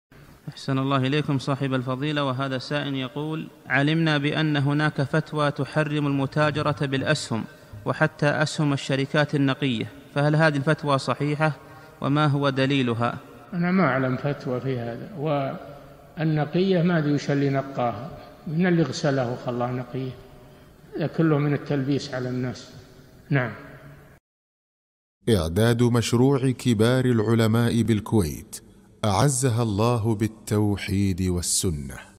[105 -560] هل يوجد في تجارة الأسهم ما يسمى بالأسهم النقية؟ - الشيخ صالح الفوزان
-"السؤال: علمنا بأن هناك فتوى تحرم المتاجرة بالأسهم، وحتى أسهم الشركات النقية، فهل هذه الفتوى صحيحة، وما هو دليلها؟
أنا ما أعلم فتوى في هذا، والنقية ما أدري ما الذي نقاها؟ من الذي غسلها وجعلها نقية، هذا كله من التلبيس على الناس.
للاشتراك في قناة الشيخ صالح الفوزان
سؤال وجواب من شرح السنة للبربهاري ( 560 ) مقطع

للاستفسار والإقتراحات والدعم
رئيس الفريق
لمشروع كبار العلماء